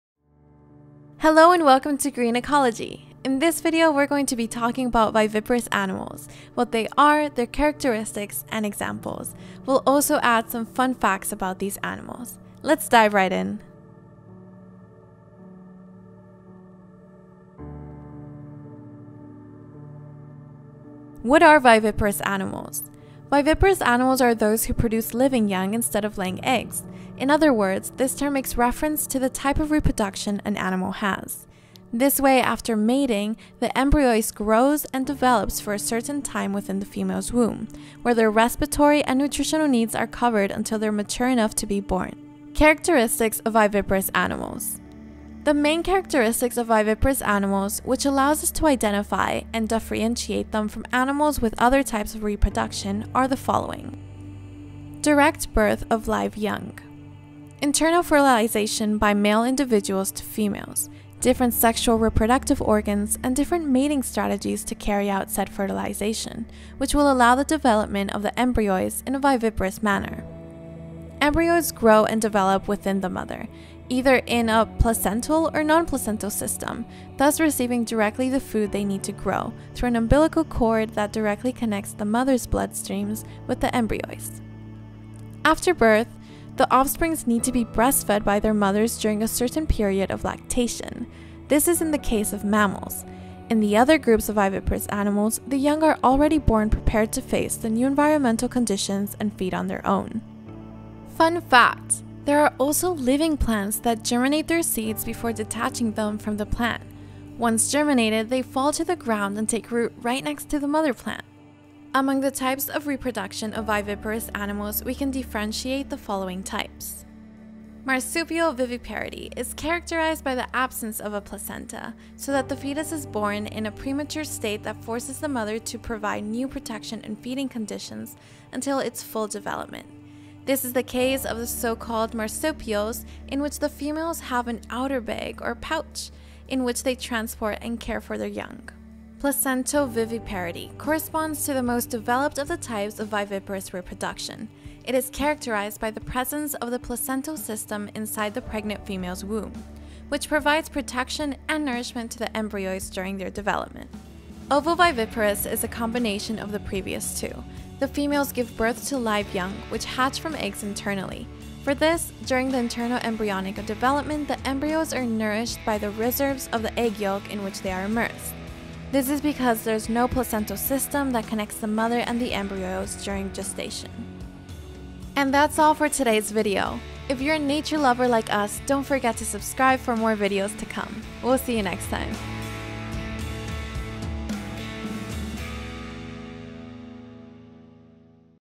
What are VIVIPAROUS ANIMALS 🐍 = 🧍 (Characteristics and examples)
🤪 HUMANS and REPTILES are LIVING! This is so, and if you want to know why, then don't miss this EcologyGreen video in which we explain what LIVING ANIMALS are and what their characteristics are.